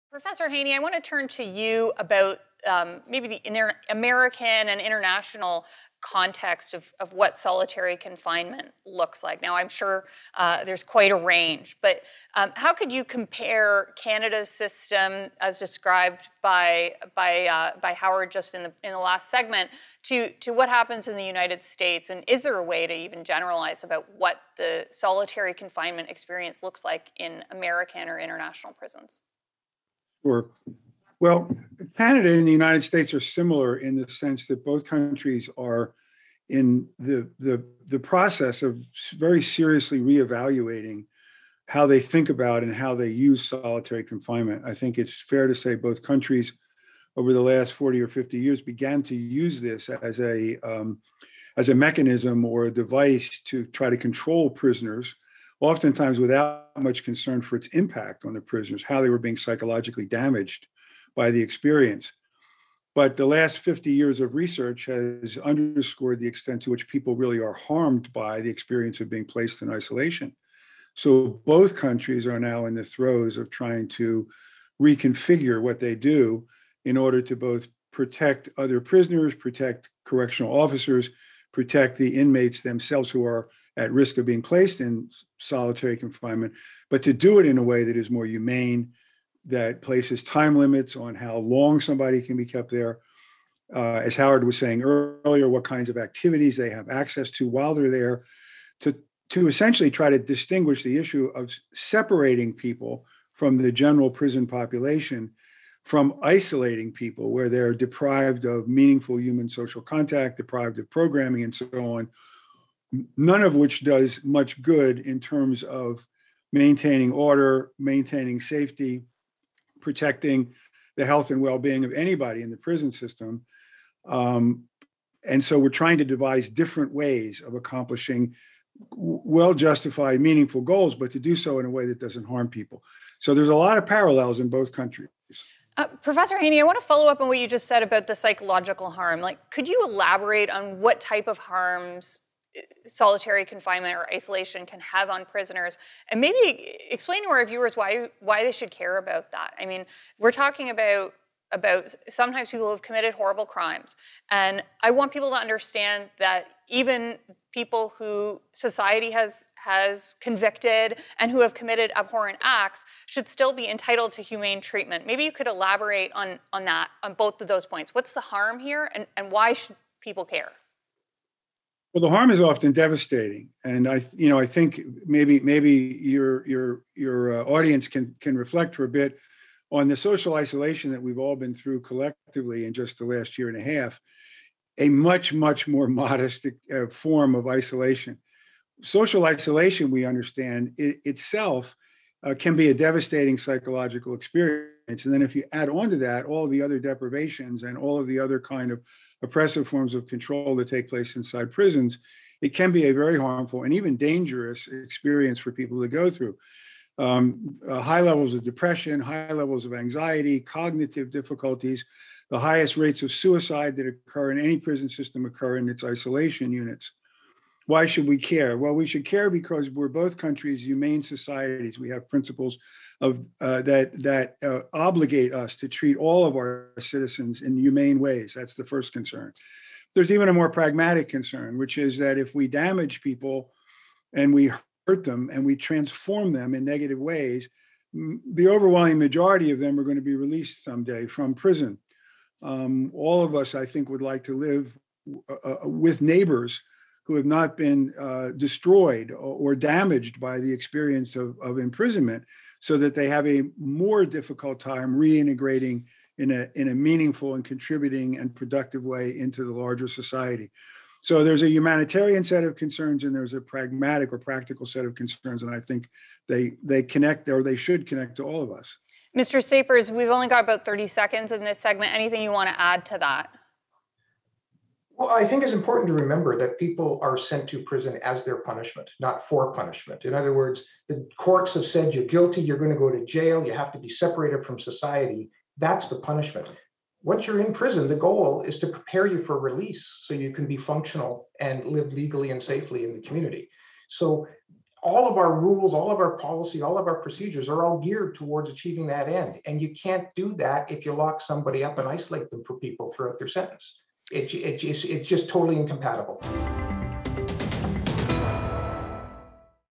Solitary Confinement in Canada vs. the U.S.
Christine and her legal panel address the controversial subject of solitary confinement, how often it is used, why it is used, and the psychological impact on prisoners.